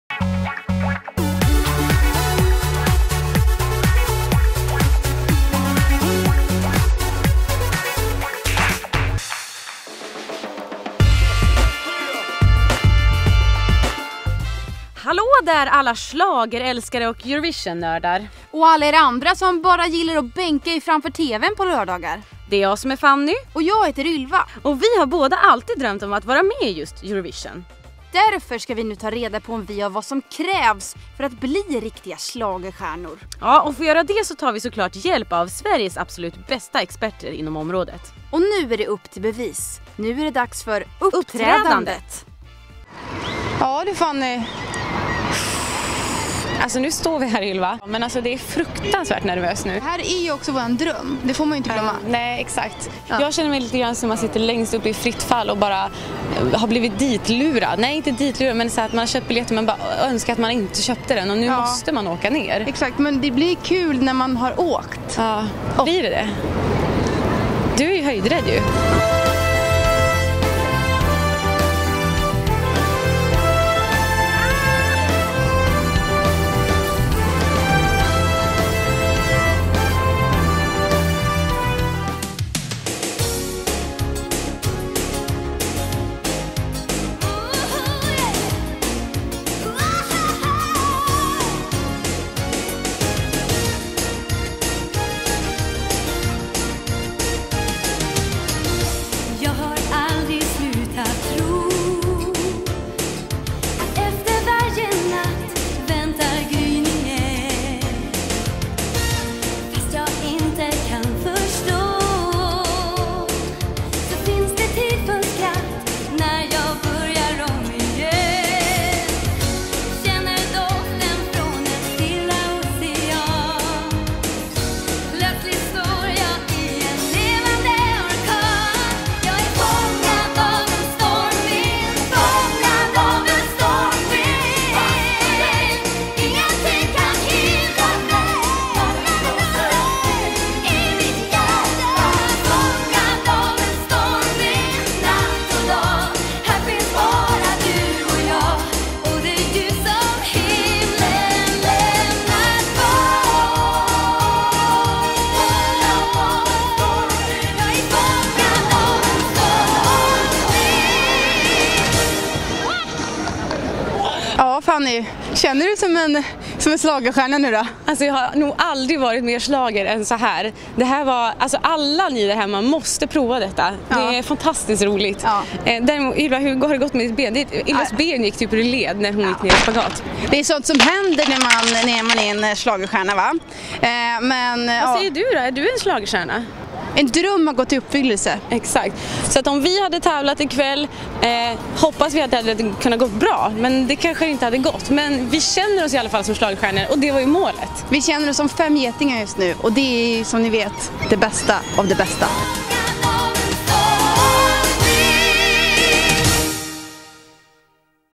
Upp till bevis: Kan man bli en schlagerstjärna?
Expressens reportrar Ylva Johansson och Fanny Forslund har under veckan förvandlats till schlagerstjärnor. Nu är det upp till bevis och dags för uppträdande på Sergels torg.